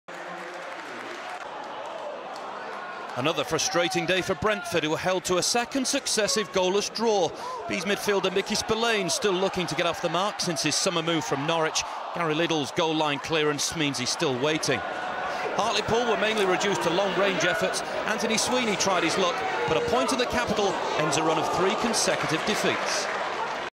Brentford 0-0 Hartlepool United - 18th September 2010
HUFC: Jake Kean, Neil Austin, Sam Collins, Gary Liddle, Evan Horwood, Colin Larkin (Leon McSweeney 69), Antony Sweeney, Paul Murray, Fabian Yantorno (Steve Haslam 69), Andy Monkhouse, Ritchie Humphreys (Denis Behan 78) // Manager - Mick Wadsworth

BFC: Ben Hamer, Kevin O'Connor, Michael Spillane, Karleigh Osborne, Pim Balkestein, Craig Woodman, Nicky Adams (Sam Saunders 65), Toumani Diagouraga, Myles Weston, Charlie MacDonald, Gary Alexander (Nicky Forster 78) // Manager - Andy Scott